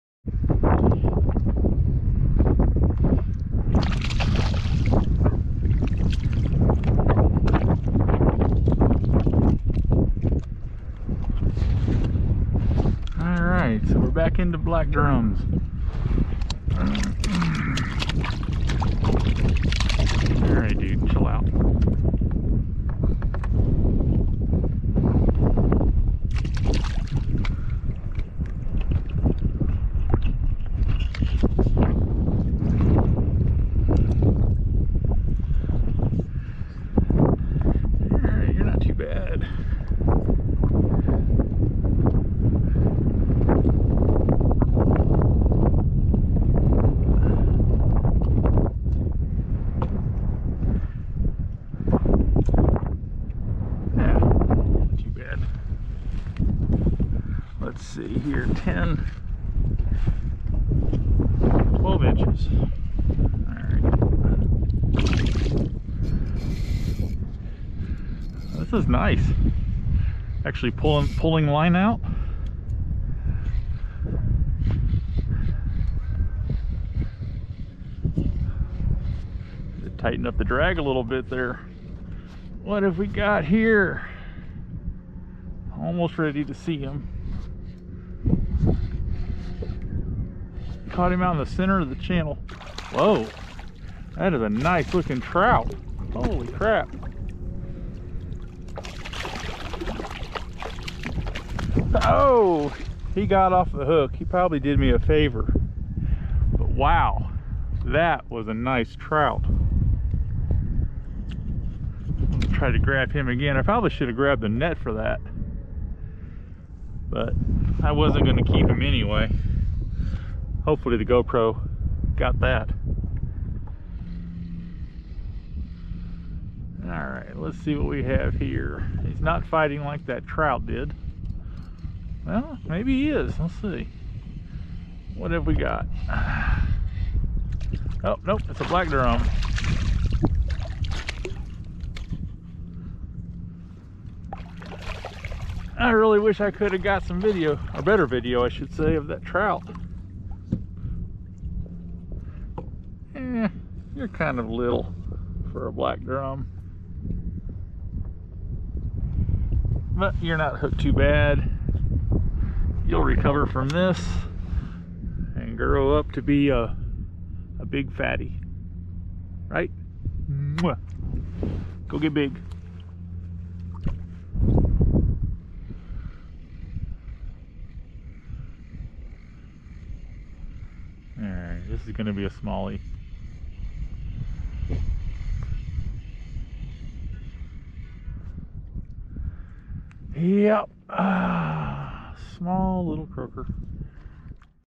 Kayak Fishing Beaver Dam Creek in Oak Island, NC
Caught a few black drum and a real nice trout